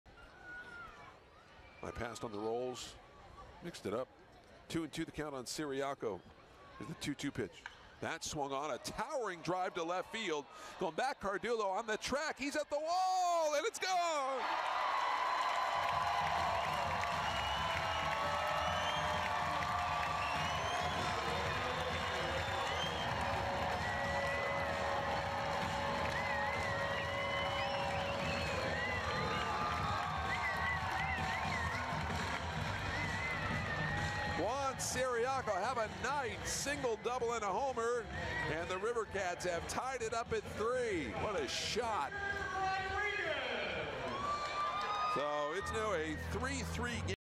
Sacramento's Ciriaco hits leadoff shot
6/6/16: Sacramento's Juan Ciriaco belts a leadoff homer in the seventh inning of a 5-3 win over Albuquerque